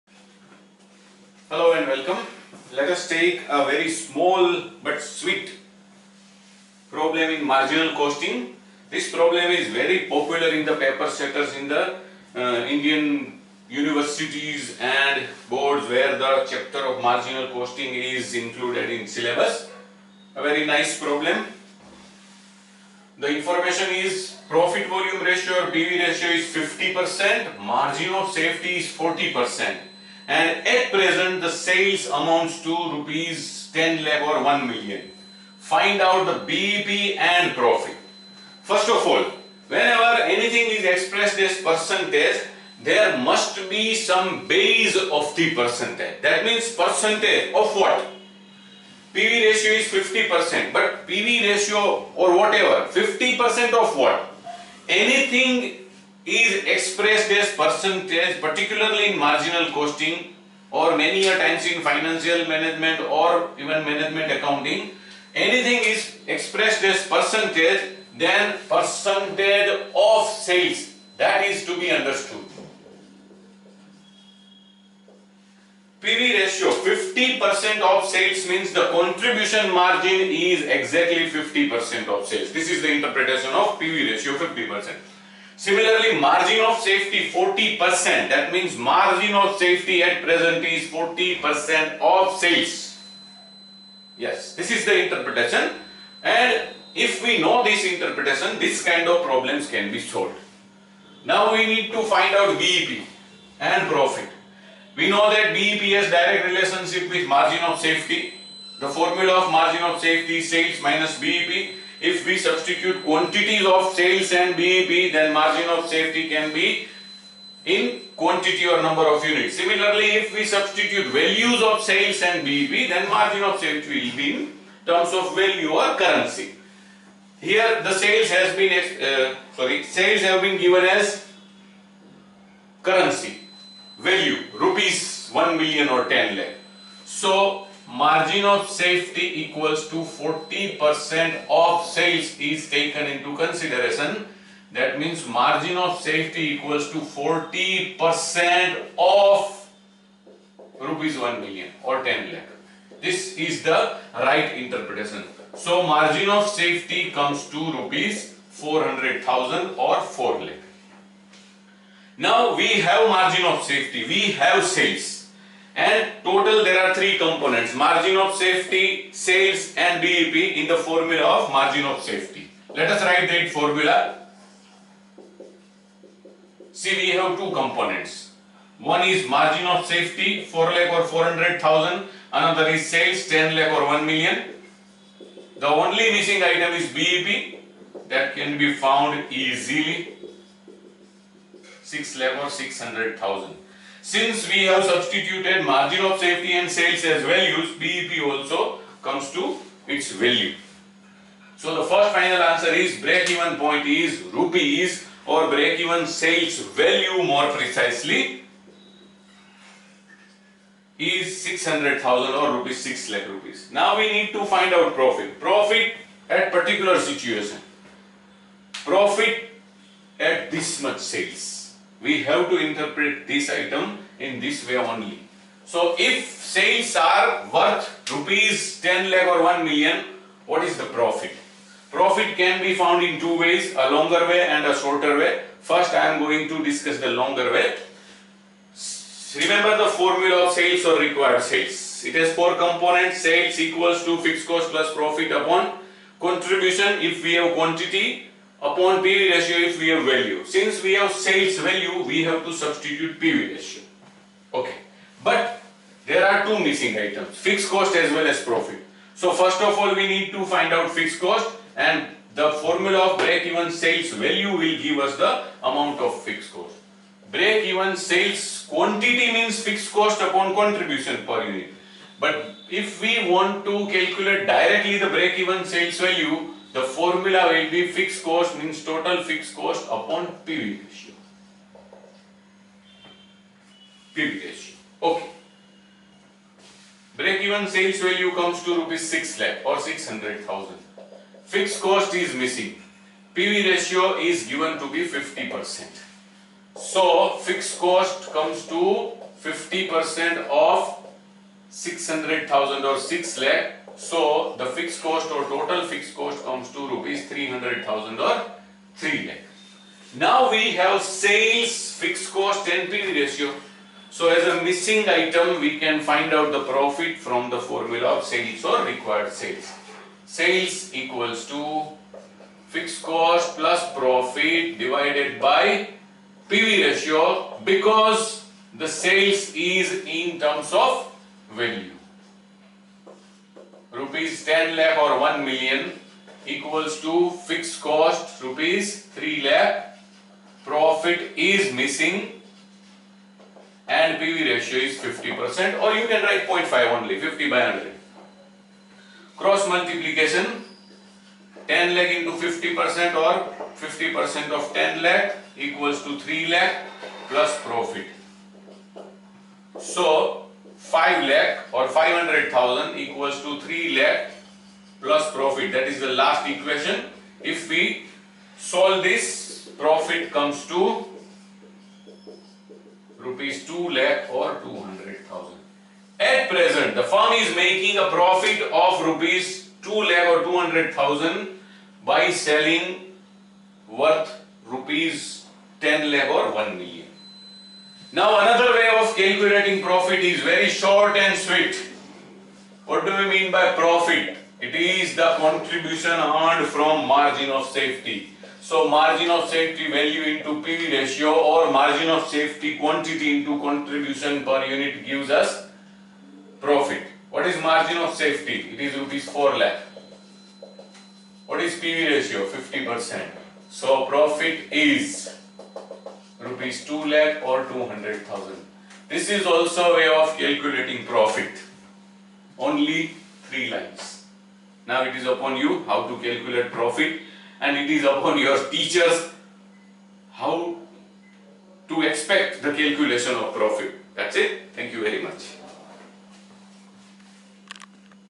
Marginal Costing 17 To find BEP and Profit from PVR, MOS and Sales
Marginal Costing - 17

Tocalculate the BEP and Profit when the information about Sales, P/V Ratio and the Margin of Safety as a percentage (of sales) is available.

Example

The P/V Ratio of a company is 50% and the Margin of safety is 40 %. Find out the BEP and profit, if the sales are Rs. 10,00,000.